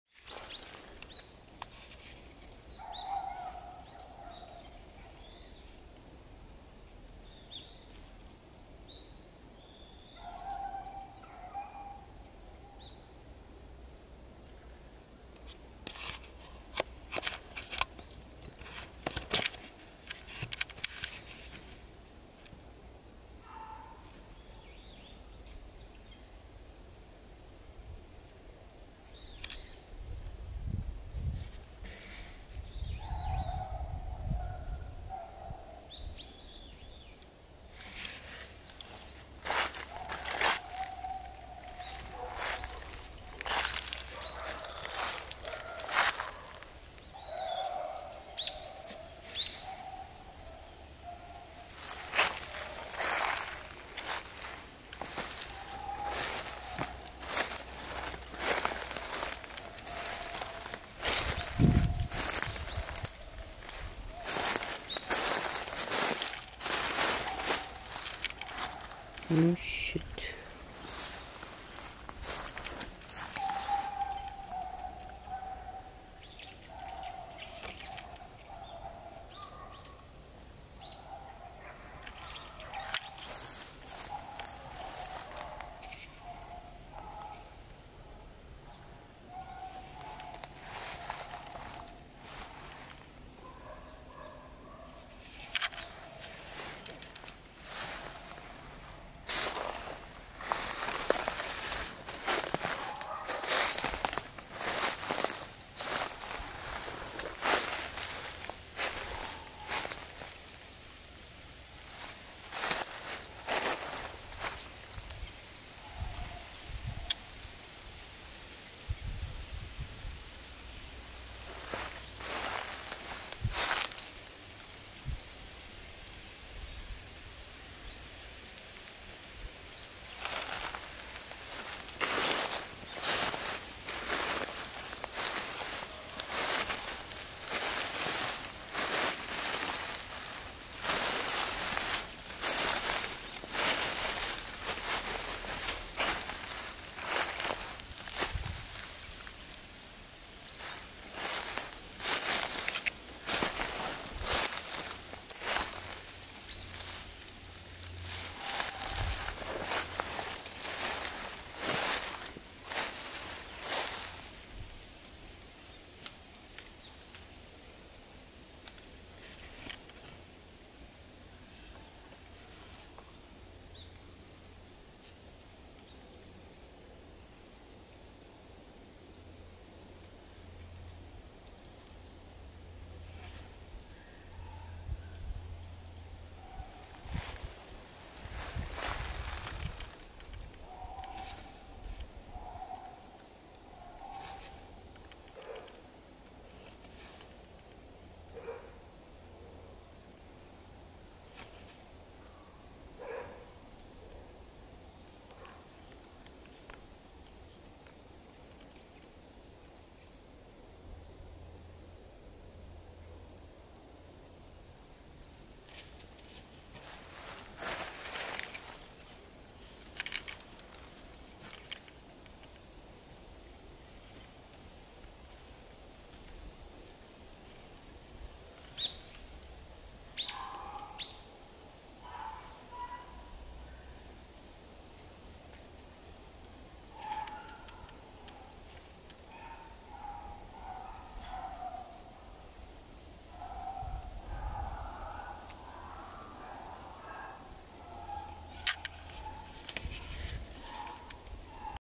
2016/11/15
A peaceful scenery of backyard in a countryside afternoon.